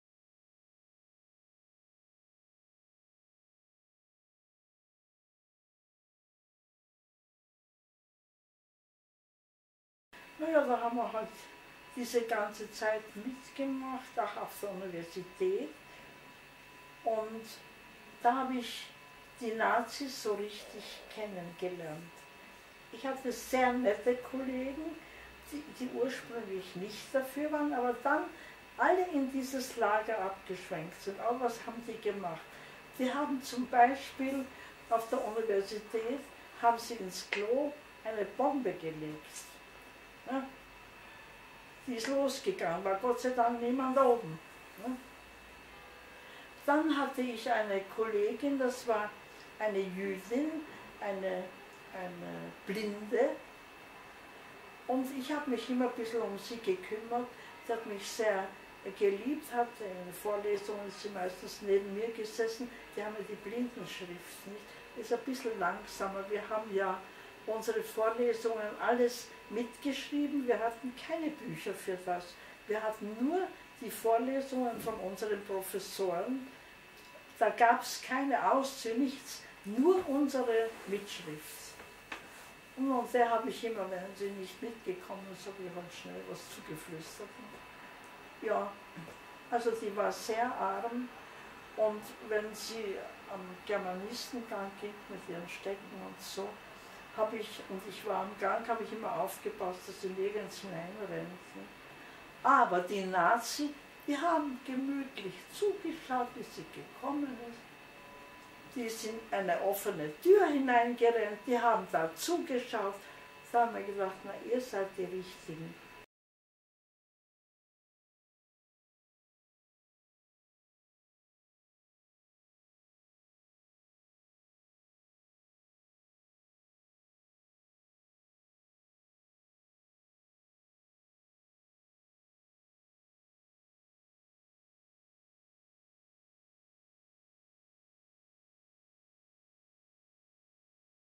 Nazis an der Karl-Franzens-Uni Graz
In den Örtlichkeiten der Universität Graz wird durch Zufall eine Bombe gefunden. Zu diesem Zeitpunkt befindet sich Gott sei Dank niemand in der Nähe.